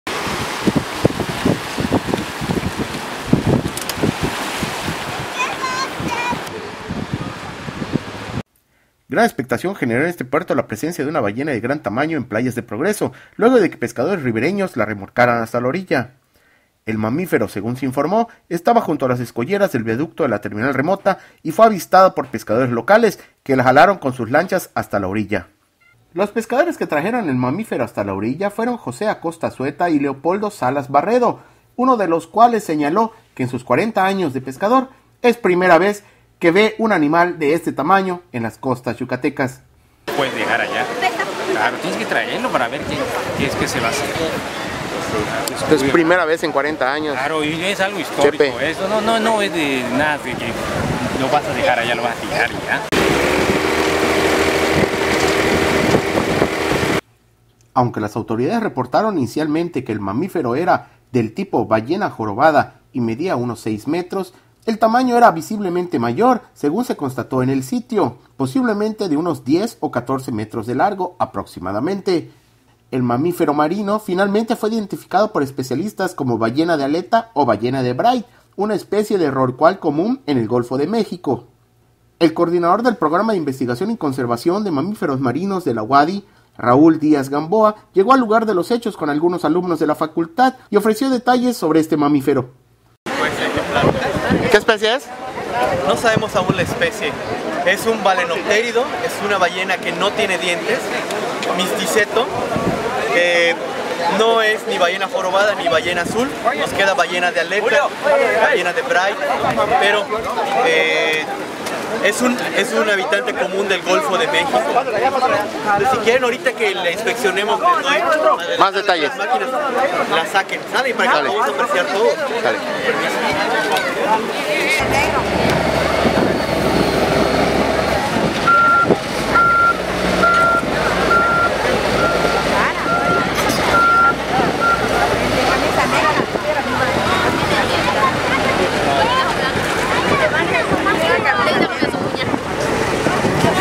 ProgresoHoy.com -- Ballena en playas de Progreso Yucatan
Ballena causa expectación en playas de Progreso Yucatan. El mamífero de gran tamaño fue remolcada a la orilla por pescadores...